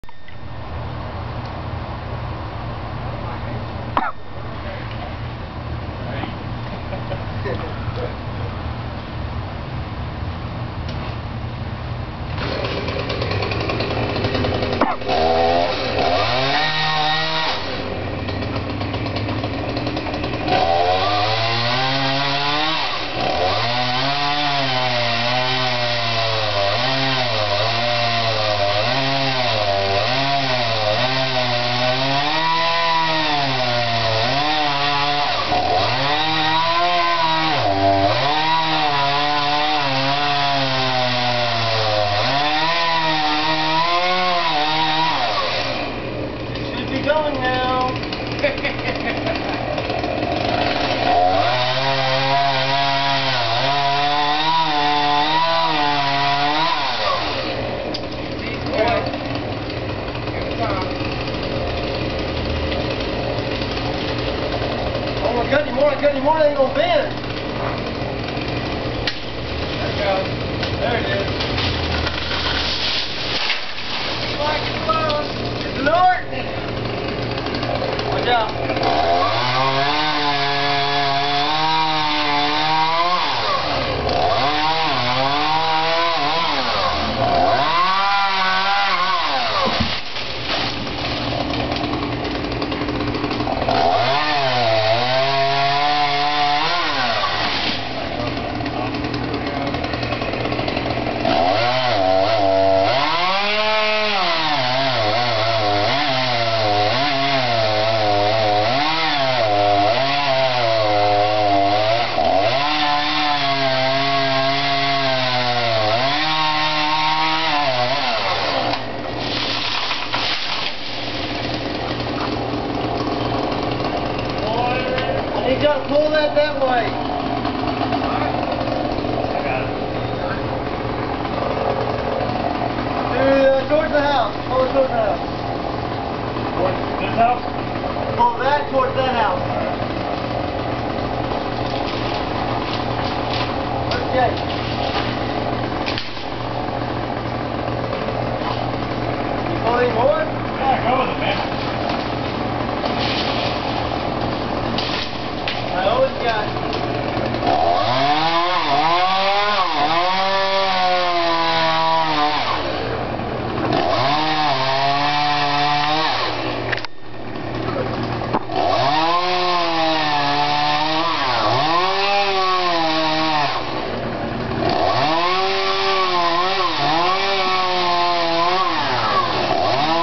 Cutting Down a Tree pt3
Tree crew cutting down my neighbor's tree